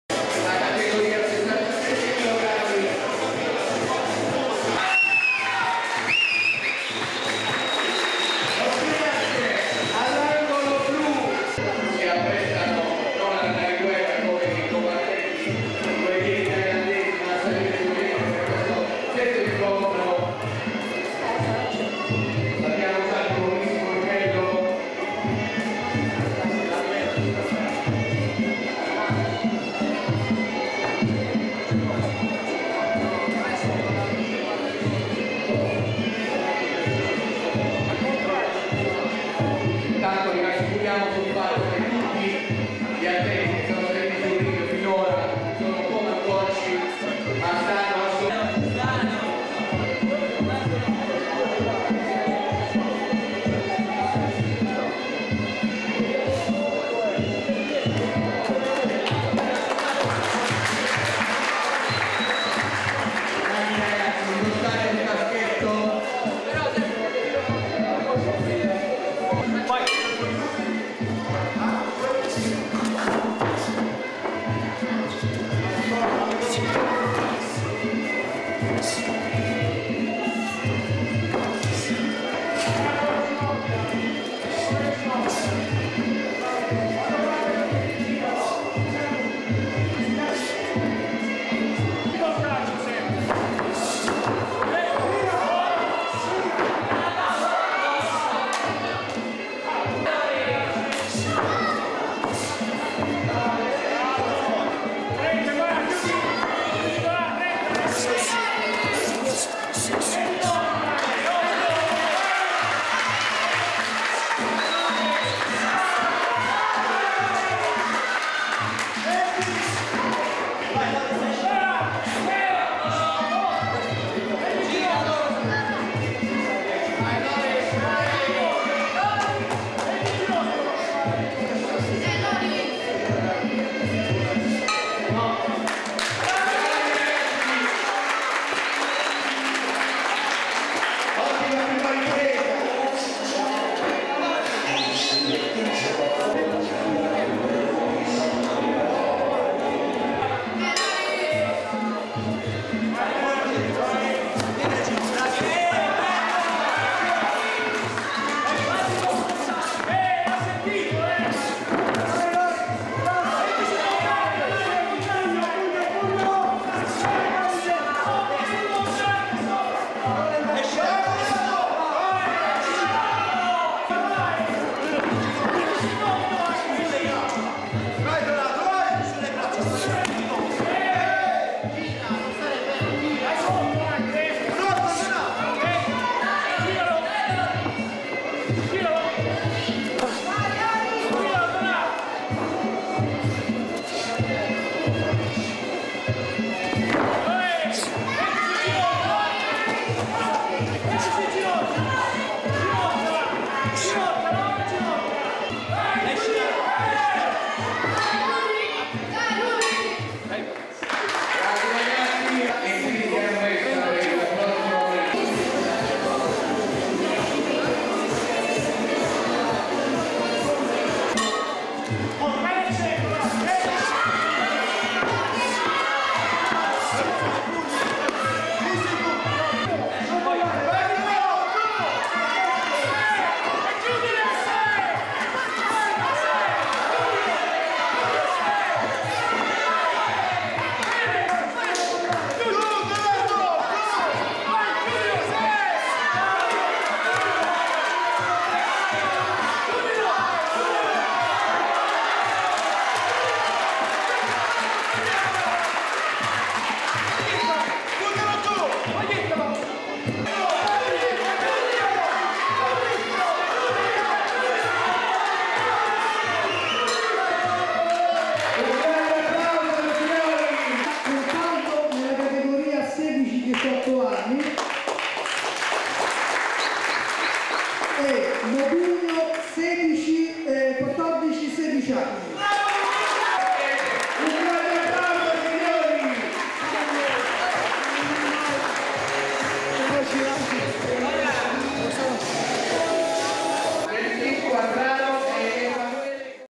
muay thai, sintesi incontro 15 giugno Loris Modugno - Donato Contaldo
La sintesi dell'incontro del 15 giugno tra Loris Modugno e Dontato Contaldo